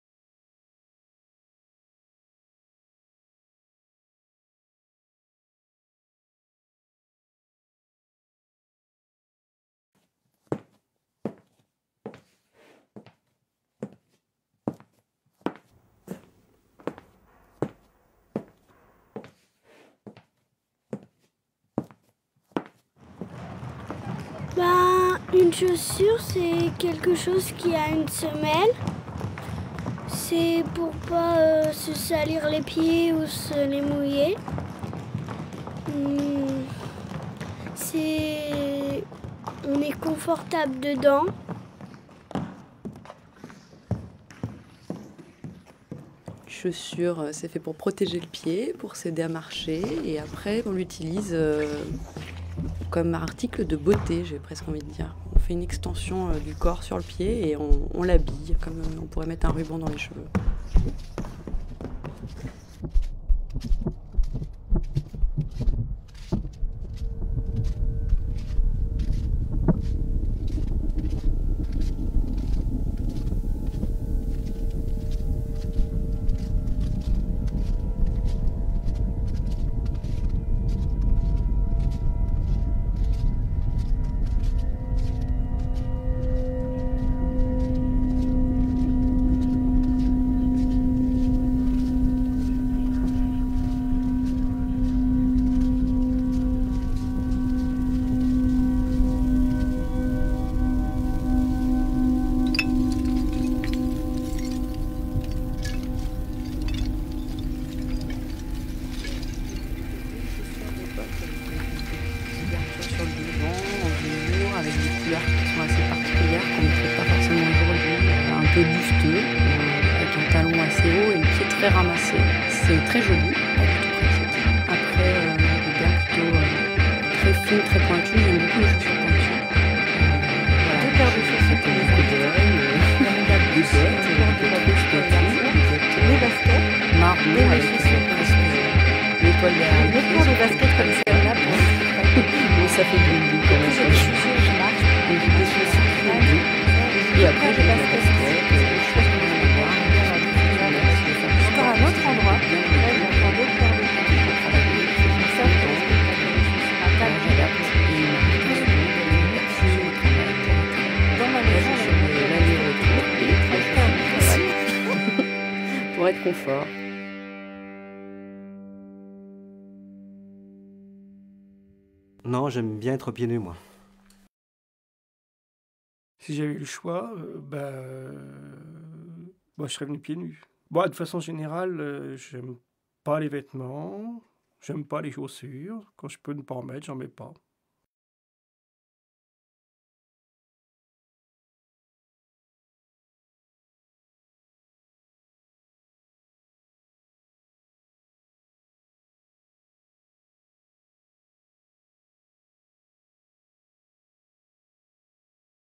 Bien dans ses pompes
Danse "d'aujourd'hui" avec des danseurs "autrement" capables